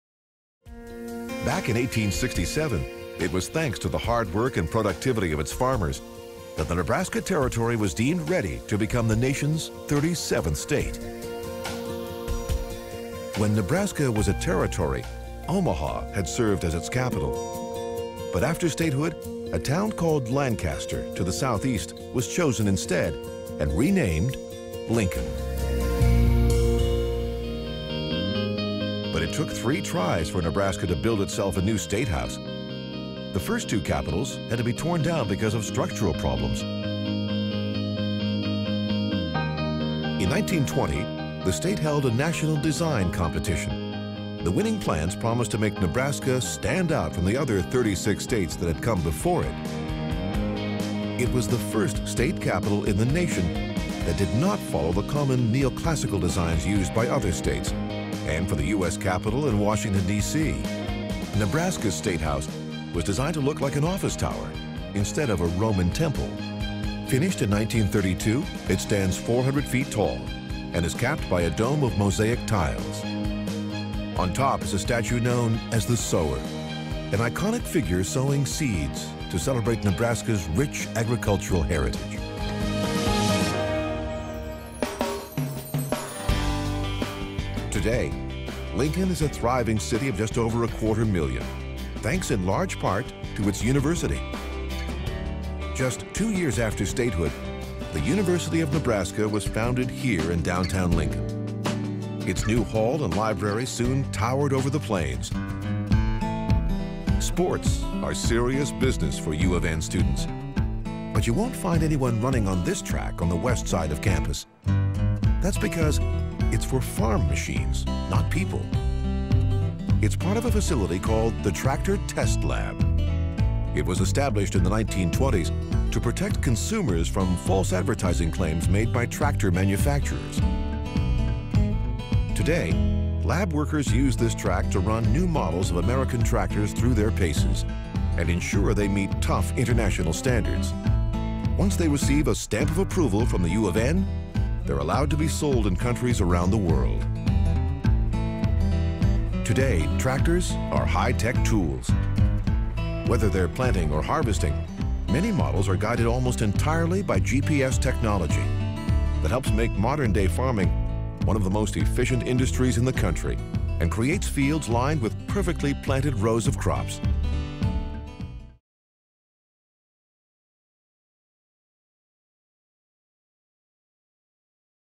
Nebraska is the Home of Reliable Tractors
When Nebraska was a territory, Omaha had served as it's capital, but after statehood a town called Lancaster was chosen instead and renamed, Lincoln.